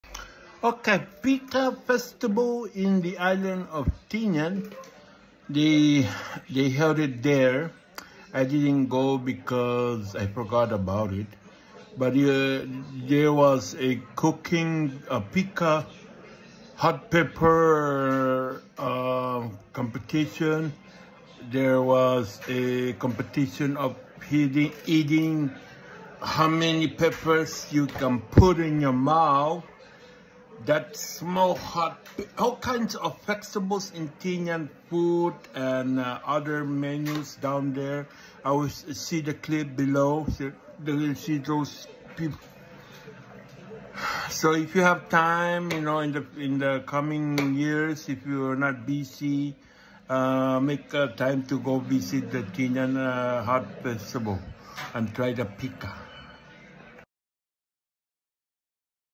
Tinian pika hot pepper festival lots of spicy 🌶️ in tinian island@JeromeLivingLife
Tinian Pika Hot Pepper Festival: A vibrant celebration on Tinian Island, where fiery spicy peppers take center stage. Locals and visitors alike gather to savor the heat, indulge in culinary delights, and revel in the lively atmosphere. From pika salsa to pepper-infused dishes, this festival ignites taste buds and celebrates the island’s bold flavors. 🌶️🔥